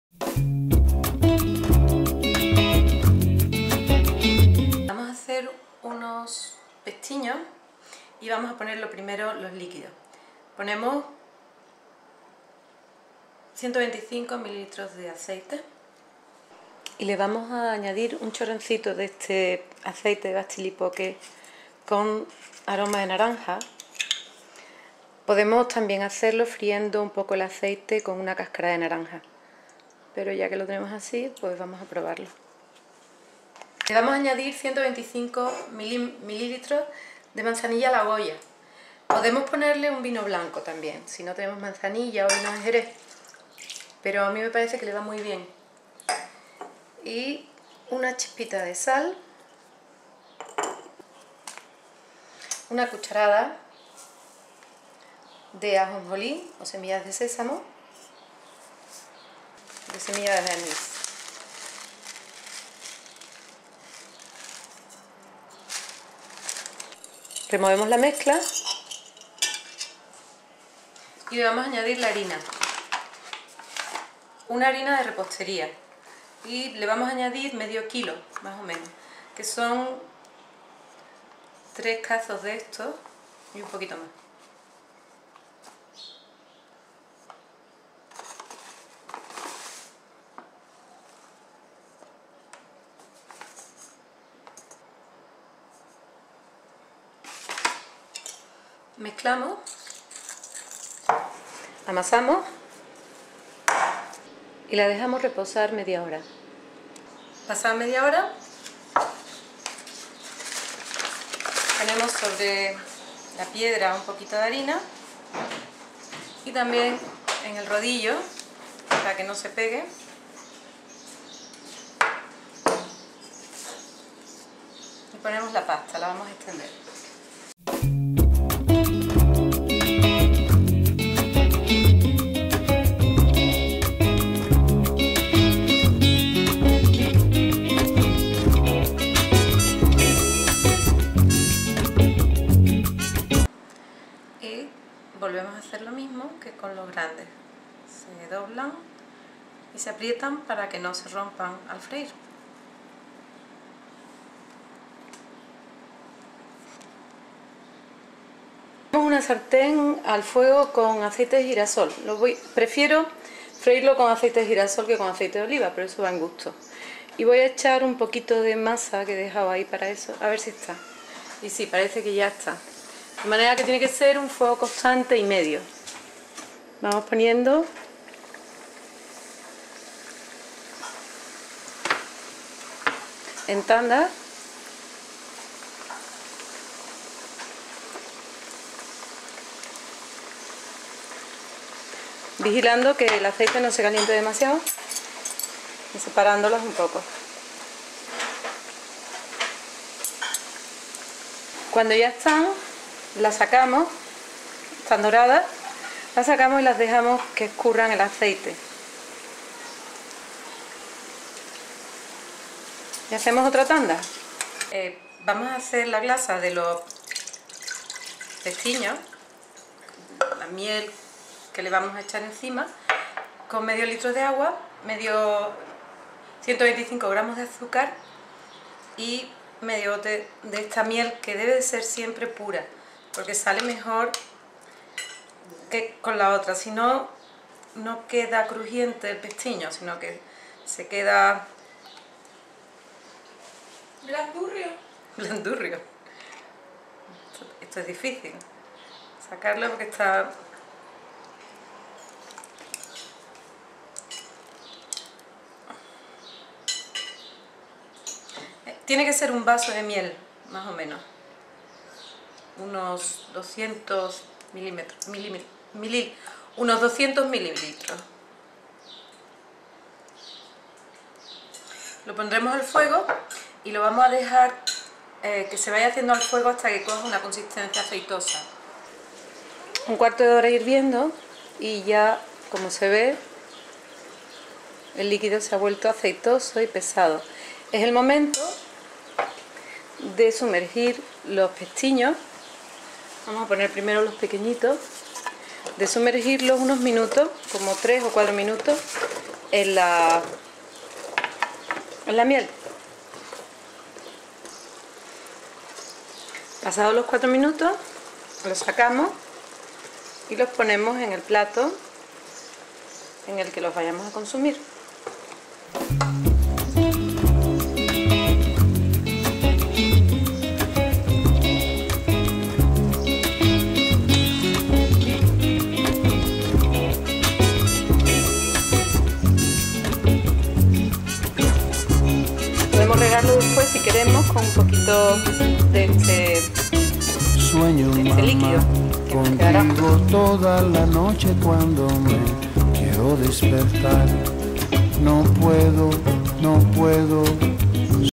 Pestiños ligeros
Ligeros y pequeños,estos dulces hechos de pasta de harina aceite de oliva virgen extra, vino de Jerez, sésamo, matalauva y miel